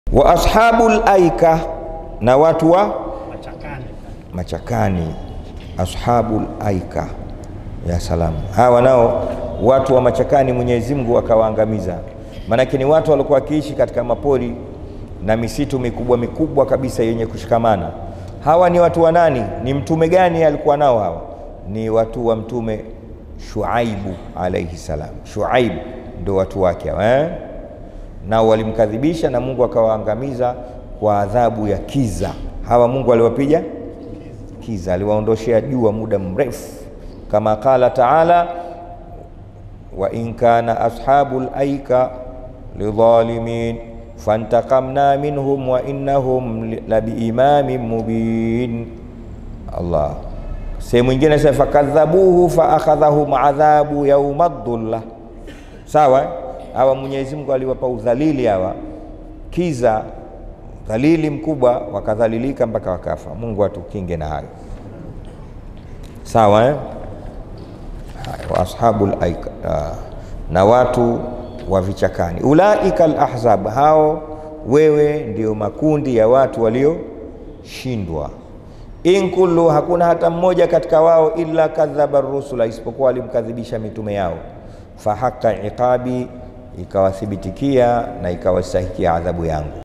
Watu Wa Machakani Waliangamizwa Kwa Kiza / Watu Wa Mtume Shuaibu Walikazibisha / Sheikh Walid
Watu  Wa  Machakani  Waliangamizwa  Kwa  Kiza / Watu Wa Mtume Shuaibu  Walikazibisha / Sheikh Walid Alhad Omar

Darsa la tafsiri kichangani 2021 la mwezi ramadhani mwaka 1442H lilotolewa na sheikh Walid Alhad  Omar kwa uzuri na ubora wa hali juu kabisa  tiizama na endelea kufatilia darsa zetu na usisahau kushare kwa wingi

And
Thanks